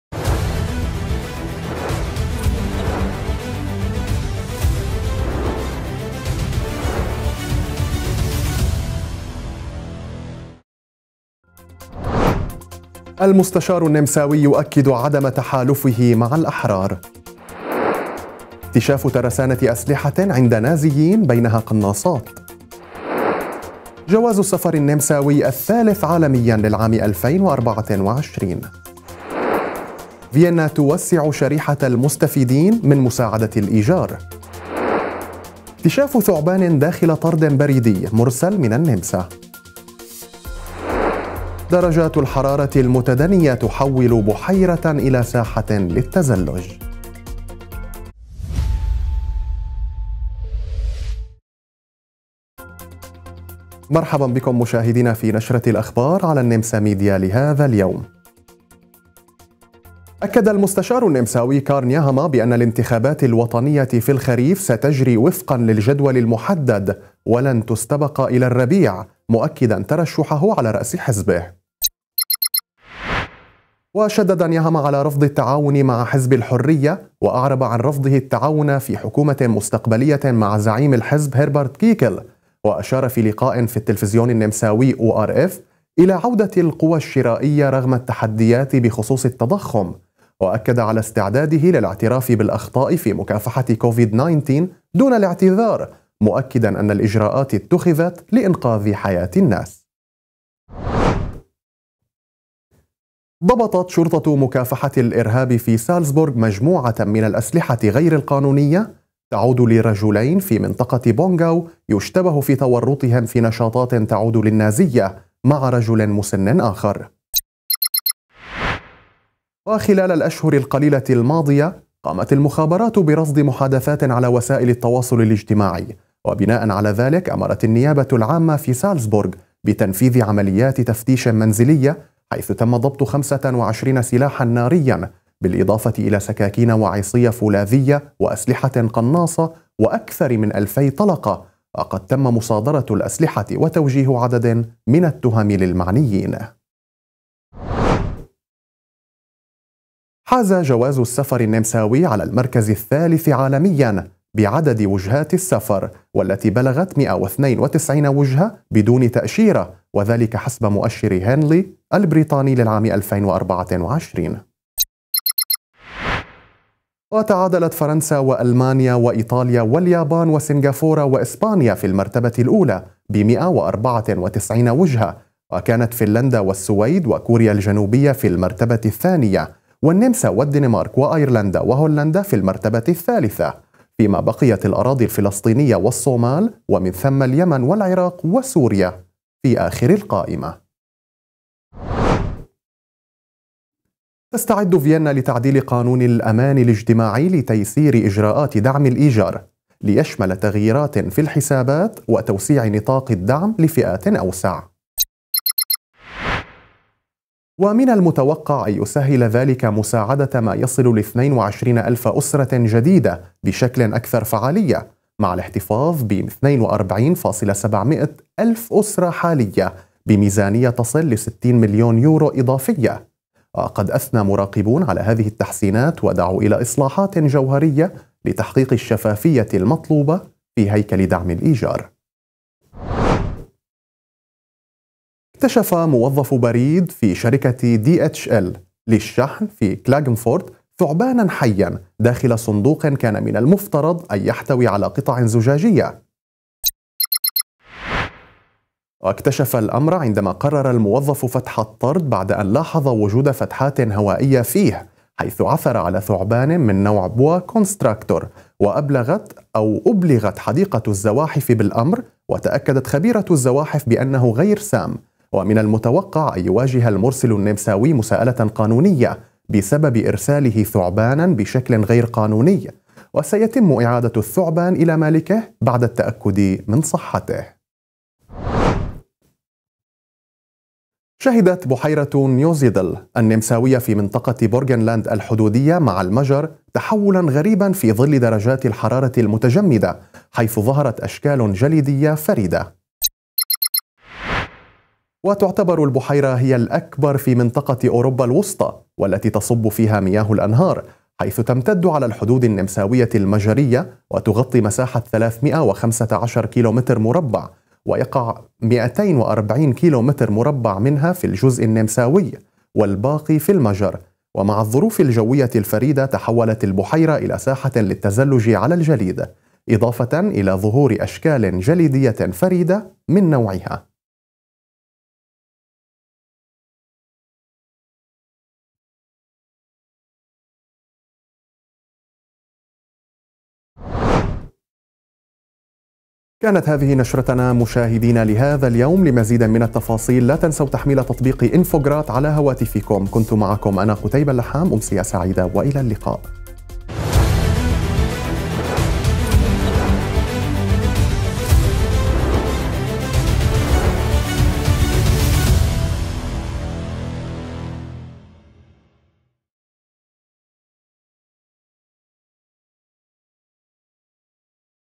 جواز السفر النمساوي الثالث عالمياً للعام 2024 وفيينا توسع شريحة المستفيدين من مساعدة الإيجار
تقديم: قتيبة اللحام